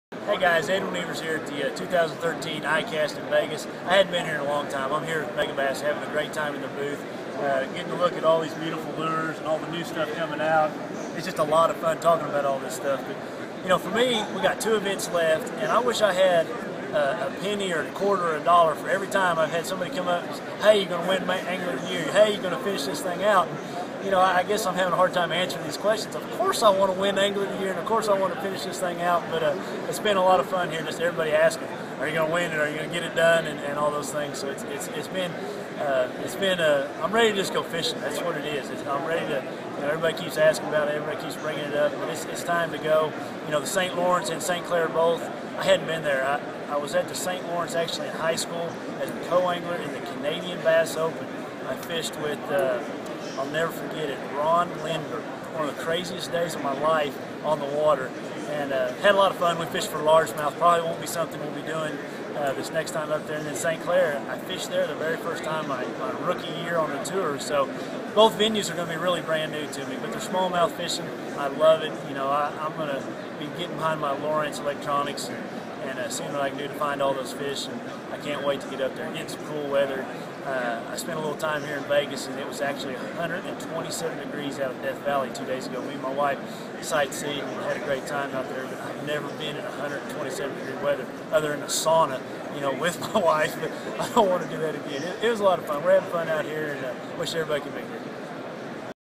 Edwin Evers on AOY, Elite Series, and Death Valley
AOY leader talks about finishing the season.